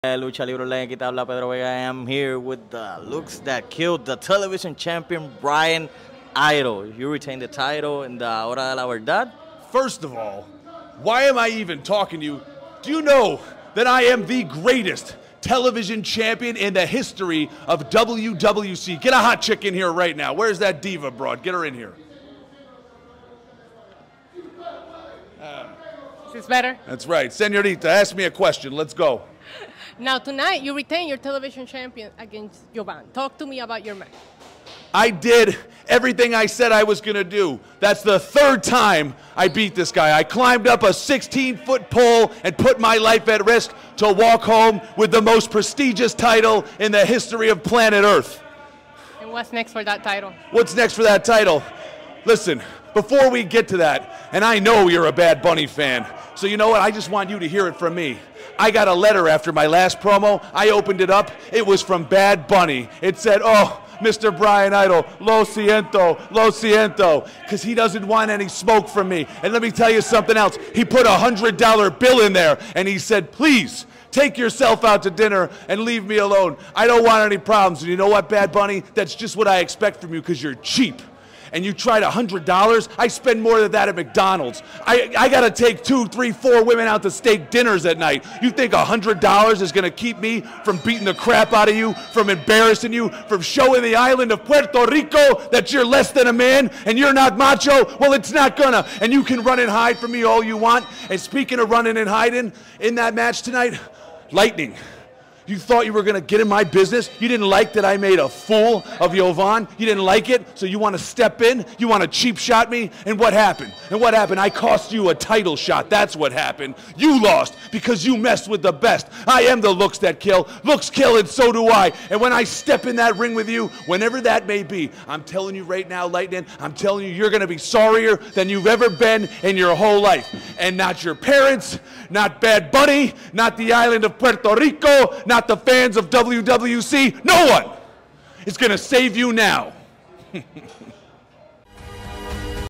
WWC TELEVISION CHAMPION BRYAN IDOL INSULTS/CALLS OUT BAD BUNNY!
WWC Television Champion Bryan Idol Insults and calls out Bad Bunny!